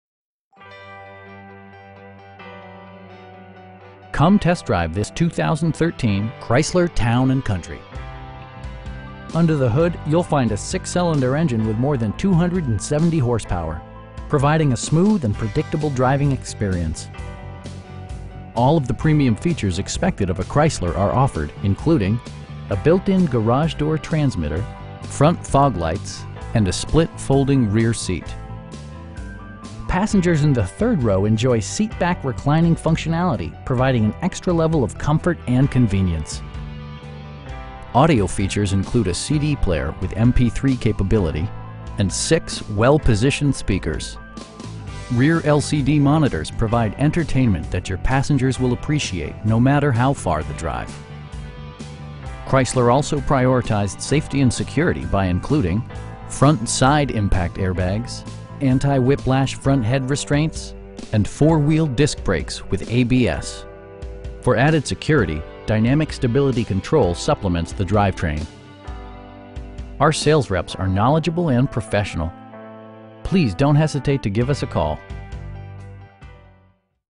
2013 Chrysler Town & Country Touring in Caledonia, MI 49316
Broadmoor Motor Group

Come test drive this 2013 Chrysler Town & Country. Under the hood you'll find a 6 cylinder engine with more than 270 horsepower, providing a smooth and predictable driving experience. All of the premium features expected of a Chrysler are offered, including: front and rear reading lights, variably intermittent wipers, a built-in garage door transmitter, front fog lights, tilt steering wheel, cruise control, and a split folding rear seat. Features such as automatic climate control and leather upholstery prove that economical transportation does not need to be sparsely equipped. Storage solutions are integrated throughout the interior, demonstrating thoughtful attention to detail. Passengers in the third row enjoy seat back reclining functionality, providing an extra level of comfort and convenience. Audio features include a CD player with MP3 capability, and 6 well positioned speakers. Rear LCD monitors provide entertainment that your passengers will appreciate no matter how far the drive. Chrysler also prioritized safety and security by including: head curtain airbags, front SIDE impact airbags, traction control, brake assist, anti-whiplash front head restraints, ignition disabling, and 4 wheel disc brakes with ABS. For added security, dynamic Stability Control supplements the drivetrain. Our sales reps are knowledgeable and professional. They'll work with you to find the right vehicle at a price you can afford. Please don't hesitate to give us a call.